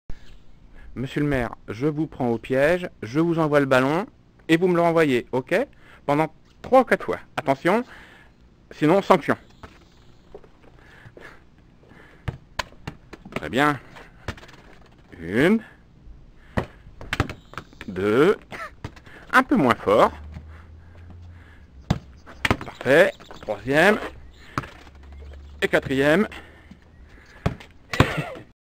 Mr le maire de La Remaudiére balle au pied
Quelques beaux intérieurs du pied droit un peu appuyés certe. mais l'esprit foot est bien là.Merci à vous Mr Coraud.